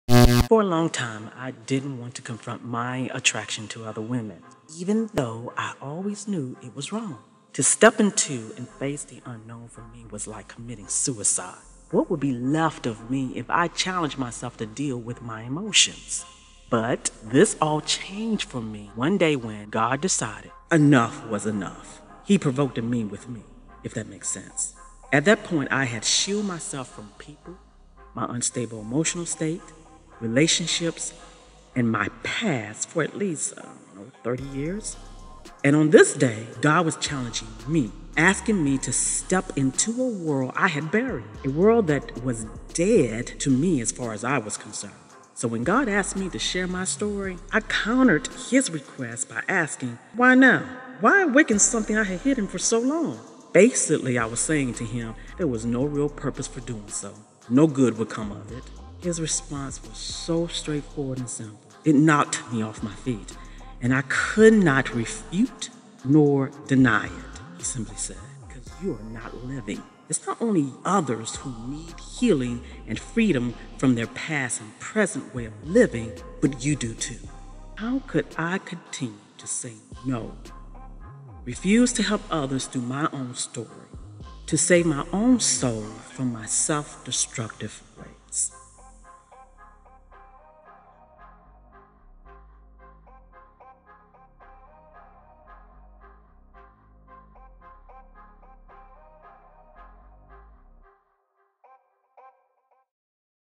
I Was Attracted to Women, but Then I Had an Encounter with God
At first, I didn’t want to address my attraction for other woman. That would be suicidal. But God provoked me by….

To hear more of my story, play my video. or my Facebook page at Surrendering: A Blueprint of My Life.

To get a copy of my book,